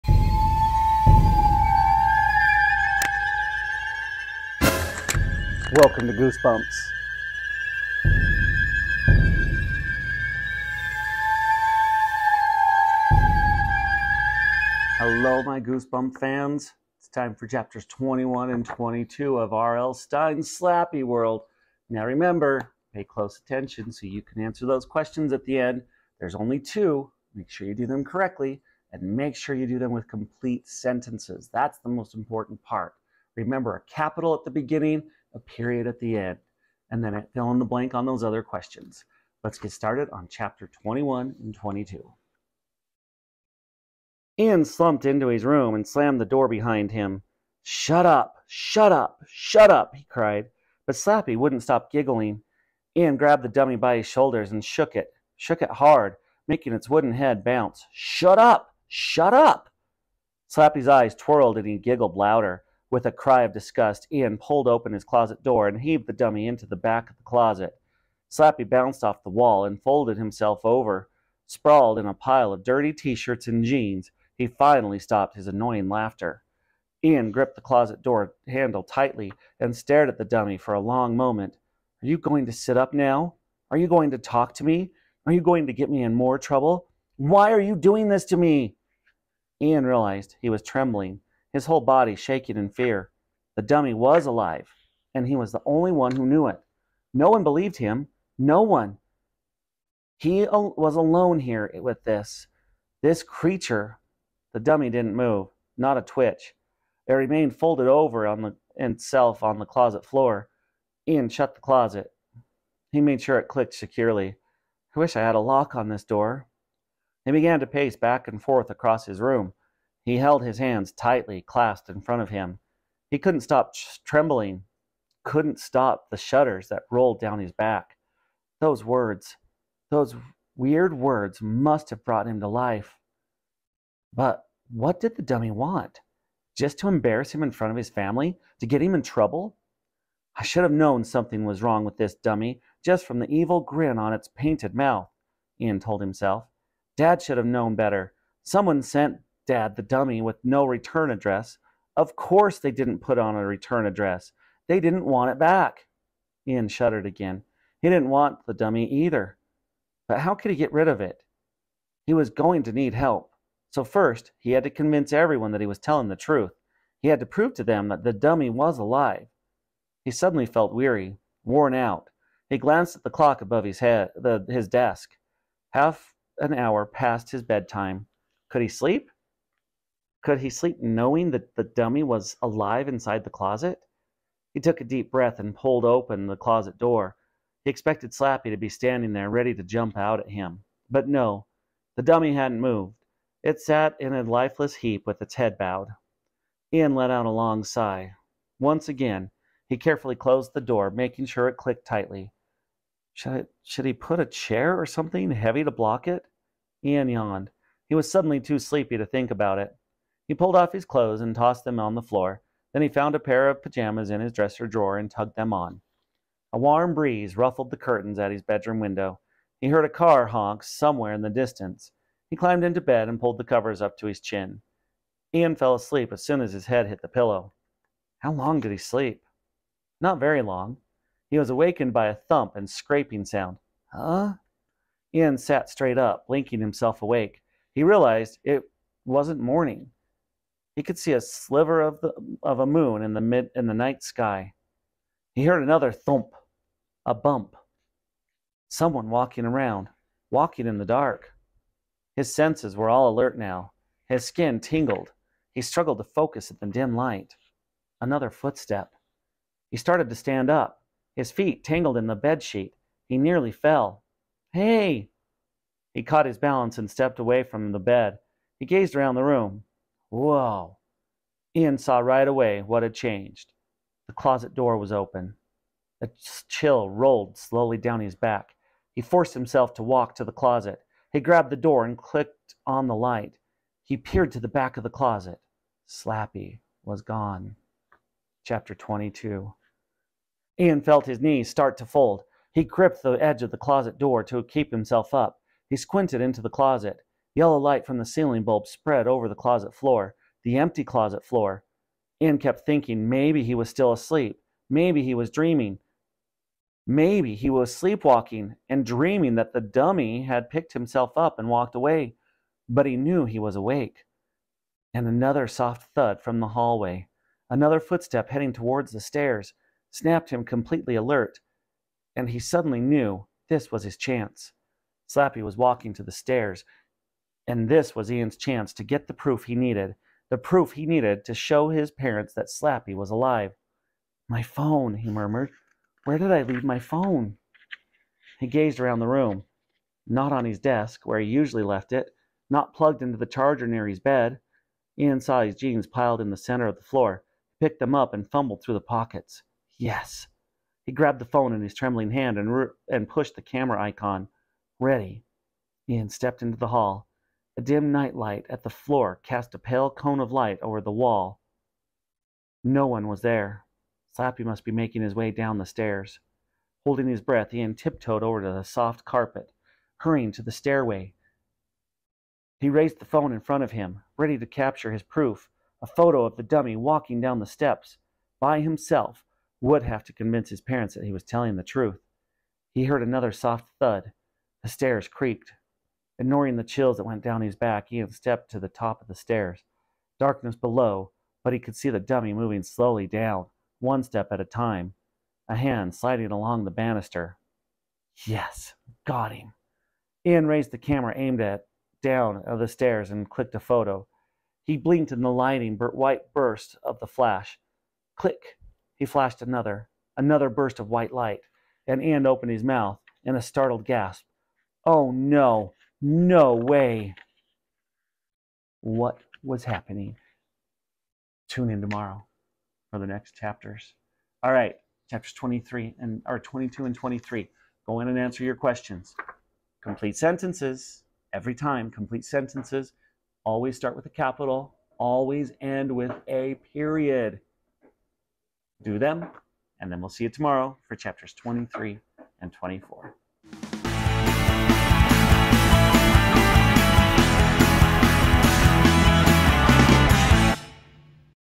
Slappy Birthday to You Chapters 21&22 HD 720p
Welcome to Behind the Bow Tie presents Story Time with Mr. Wilkes! 📚🎩 Join us for a thrilling reading session from R.L. Stine's Goosebumps: Slappy World - "Slappy Birthday to You." Get ready for spine-tingling adventures and mysterious tales brought to life by Mr. Wilkes.